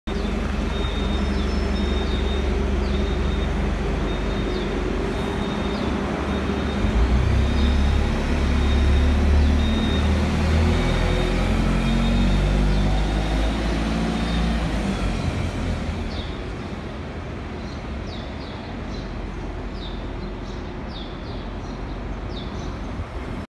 大台南公車/豐榮客運，車號試-0321沙崙環狀自動駕駛實驗車中通客車，為個人首拍台南版中通客車
110/2/18 大台南公車台鐵沙崙站高鐵台南站拍攝的大台南公車/豐榮客運，車號試-0321沙崙環狀自動駕駛實驗車中通客車，為個人首拍台南版中通客車